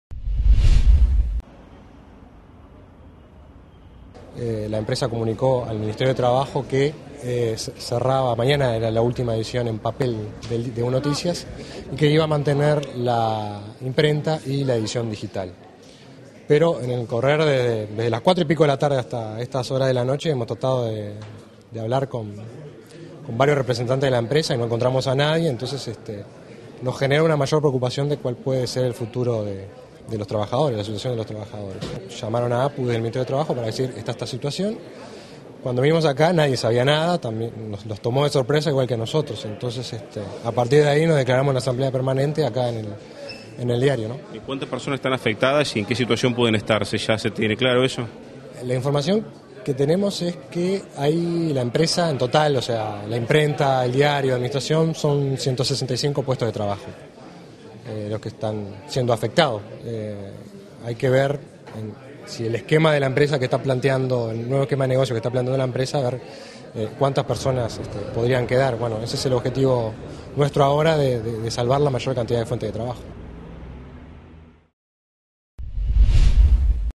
Cierra el diario UNoticias; trabajadores decidieron ocupar
La empresa emitió un comunicado en el que señala que "deja en suspenso" la edición física del matutino. Un centenar de trabajadores irá al seguro de paro.